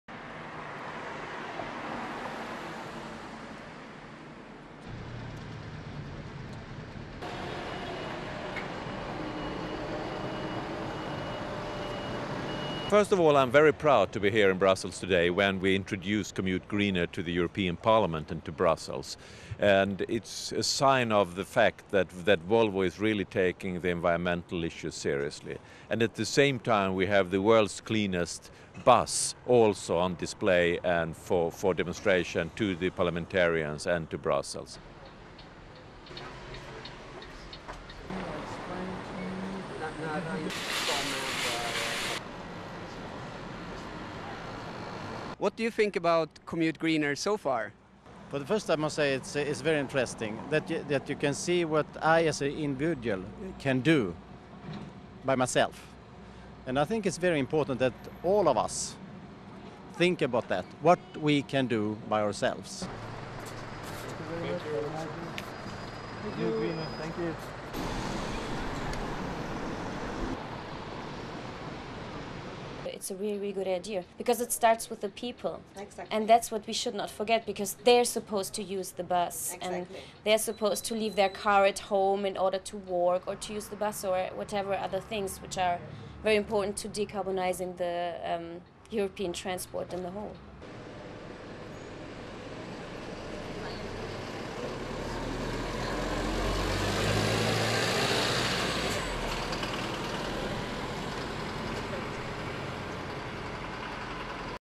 Commute Greener introduced to Bruxells and the European Parliament
Jan-Eric Sundgren, SVP Volvo Group
Olle Ludvigsson, Member of European Parliament
Cezara Petruc, Assistant to MEP Transport and Tourism

Magnus Kuschel, Managing Director Commute Greener!